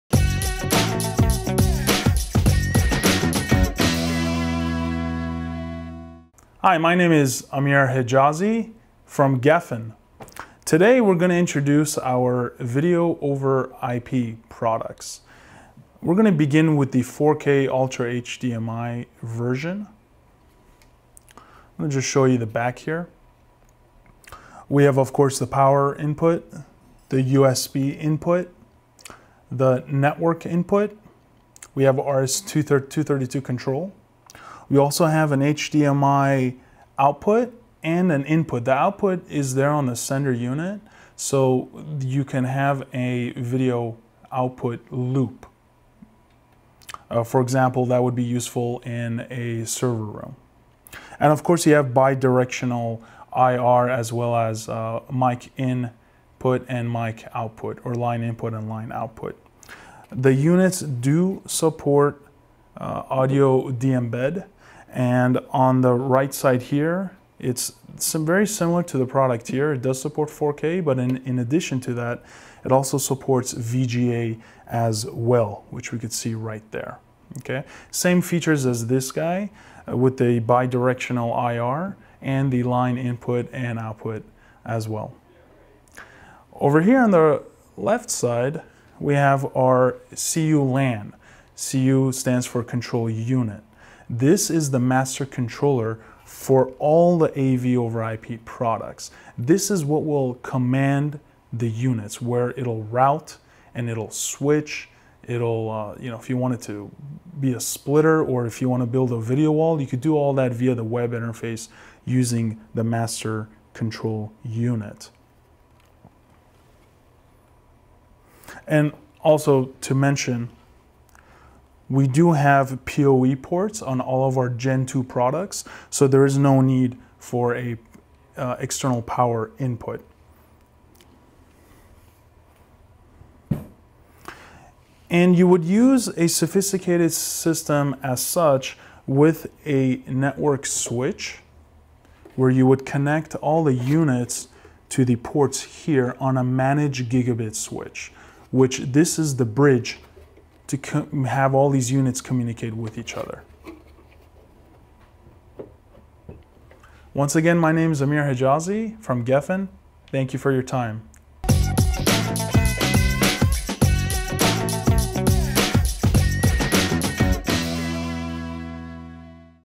New Gefen AV Over IP Solutions
Now with support for 4K Ultra HD, Gefen AV over IP products enable scalable multi-source, multi-input video distribution systems with HDMI, DVI, or VGA. The video format can be automatically converted to the desired output type. The signal can be extended throughout the IP network, and can be accompanied by Bi-directional IR, RS-232, and USB for KVM over IP applications.

Access and Control Video Anywhere over the Network - Gefen makes it possible!

Gefen delivers a robust selection of Video over IP products:
HDMI
Extend and Distribute 4K HDMI, VGA, USB, RS-232, IR, and 2-way audio over a Local Area Network
DVI
Extend and Distribute DVI, USB, RS-232, IR, and 2-way audio over a Local Area Network
VGA
Extend and Distribute VGA, USB, RS-232, IR, and 2-way audio over a Local Area Network
Matrix Controller
The Gefen Matrix controller enables automatic detection and configuration for Gefen Video over IP and KVM over IP products. Pushbutton and web-based control allow full emulation of a standard AV matrix.